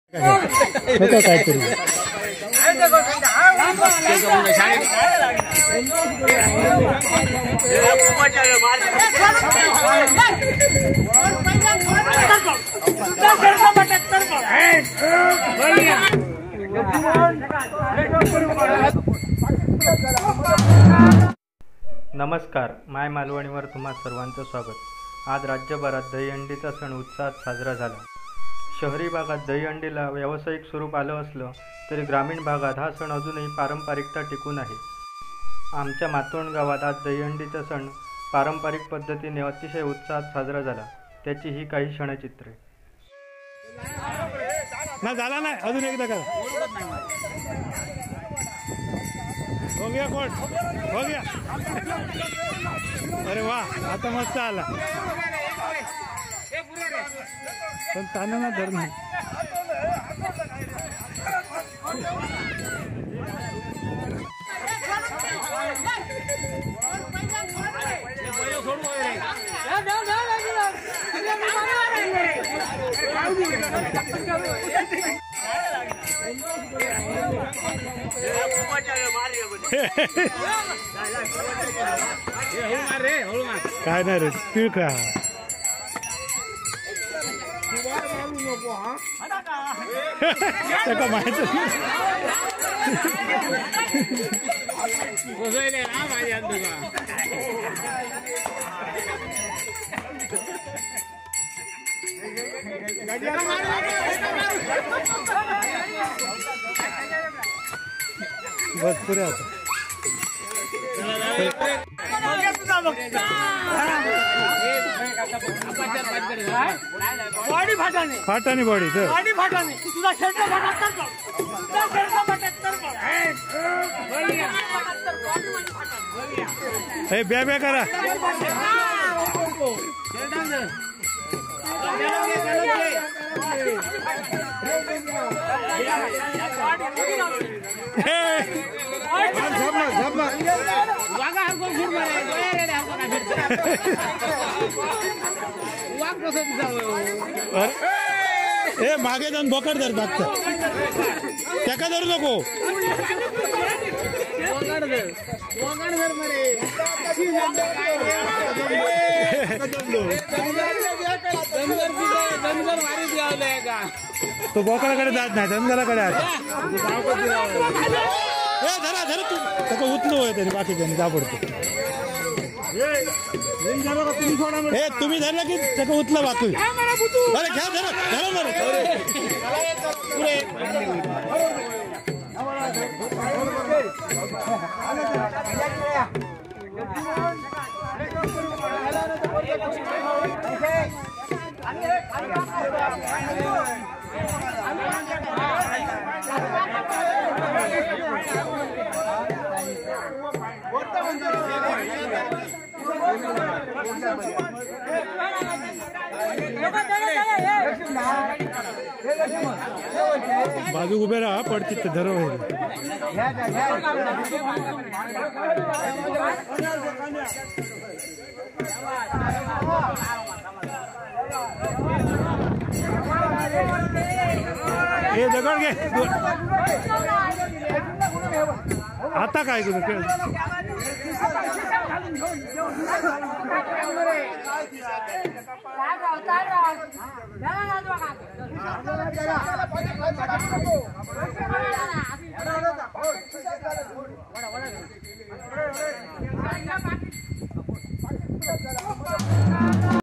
Dahi Handi| कोकणातील पारंपरिक दहीहंडी| गोपाळांचे सुंदर खेळ | हरिनाम सप्ताहाची समाप्ती| Matond
दहीहंडीच्या सणाला शहरांमध्ये व्यावसायिक रूप आलं असलं तरी कोकणात हा सण पारंपरिकता टिकवून आहे. आमच्या मातोंड गावातील रामेश्वर मंदिरात साजरा झालेला दहीहंडीचा सण. यावेळी गोपाळांनी खेळलेले पारंपरिक खेळ आणि हरिनाम सप्ताहाची सांगता. (व्हिडीओ सौजन्य- गावकर मंडळी आणि ग्रामस्थ मातोंड) @mymalvani